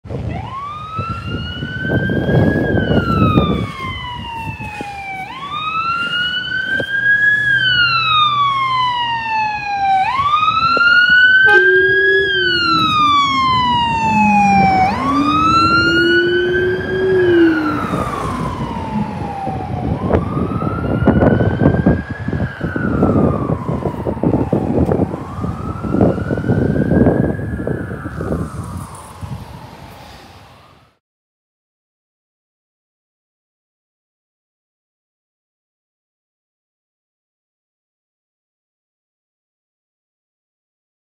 (RUMBLER) QFRS 649A 1483QF Responding To Pine Mountain Grassfire
At 2:18pm QFRS 649A 1483QF 644A 1203QF Got Toned Turnout To Pine Mountain Grassfires + Responded From Brassall/Karana Downs Fire Station At 2:20pm & 49A Used Wail Rumbler While Approaching Main Road Towards Motorway